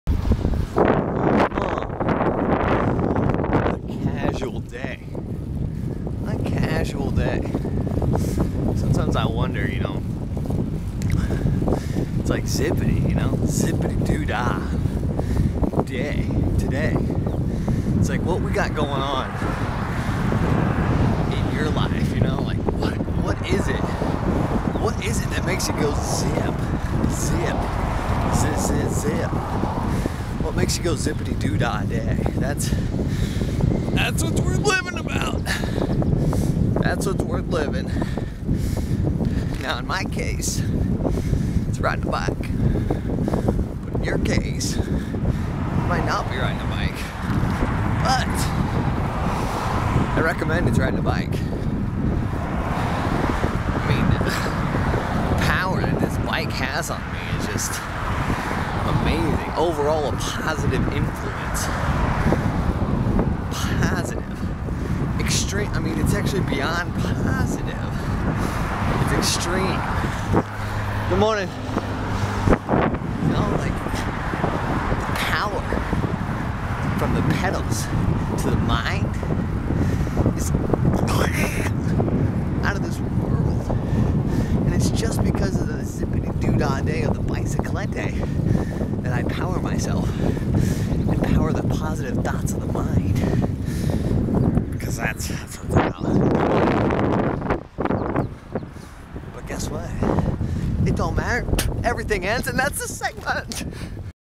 Rambling on a Bike - Intimate Thursdays: Zip, Riding a bike, Positive Thoughts
Tried bring the camera a littler closer and talking about bike riding. It is a great form of exercise! Love It! My Top Bike Products:

My Top Bike Tools For My Garage:

23 Piece Bike Tool Kit